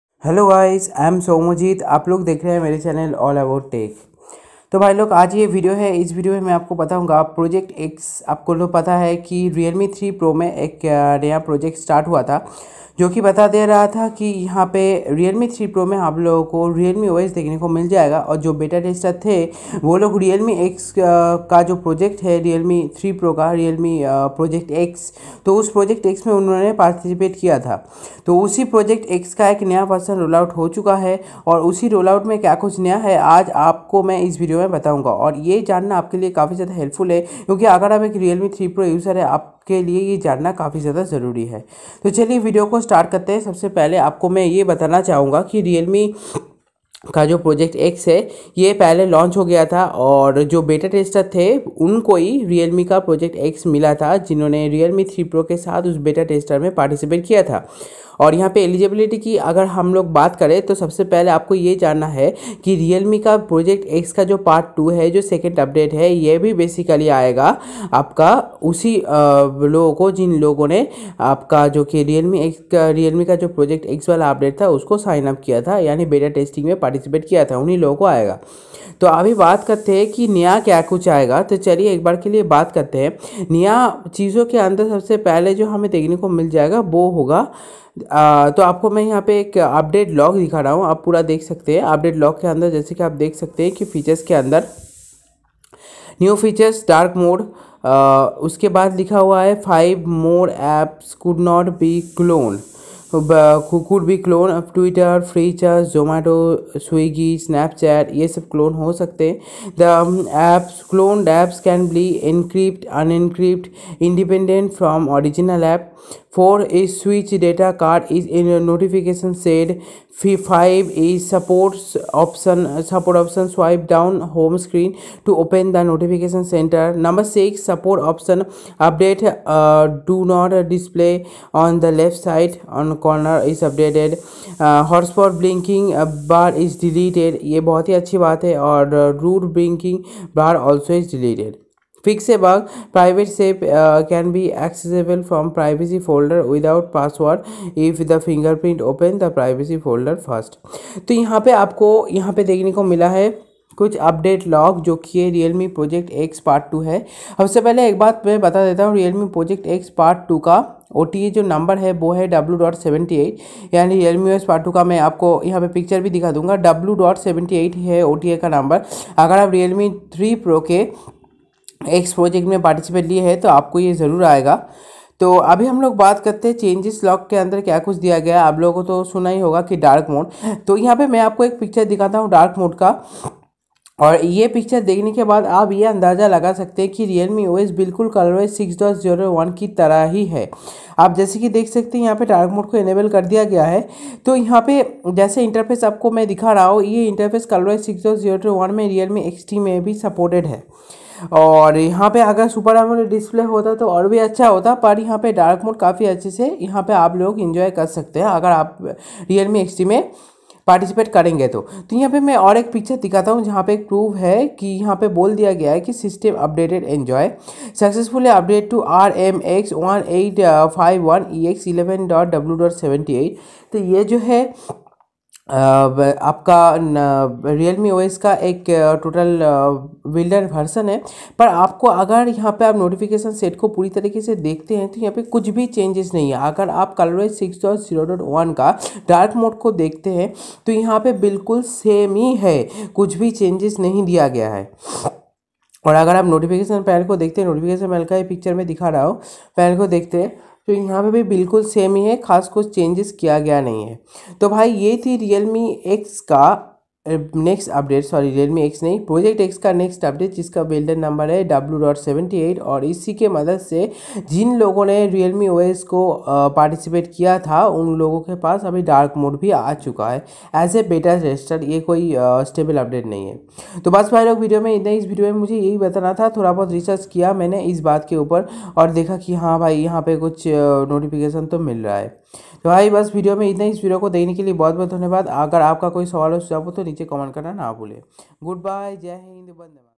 Realme OS update | Project X part 2 | Changes log
About this video:

Hello dosto, iss video ma app ko bataunga Realme OS ka 2nd update k bara ma. iss update ma app ko dark mode and clone apps and baki aur bhi apps ka support milaga ..
So lets gets started

Thank you,